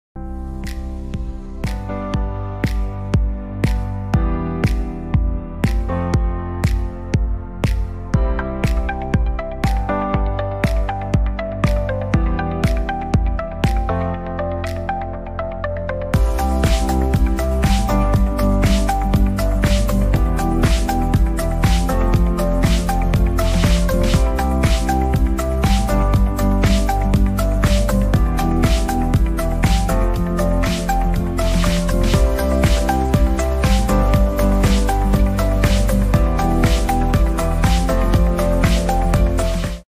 Expanded Polypropylene Foam Automatic Hot Plate Bonding Machine
This video shows how to bond expanded polypropylene foam protective packaging with our Automatic Hot Plate Bonding Machine. High efficiency, strong bonding, neat alignment, and simple operation.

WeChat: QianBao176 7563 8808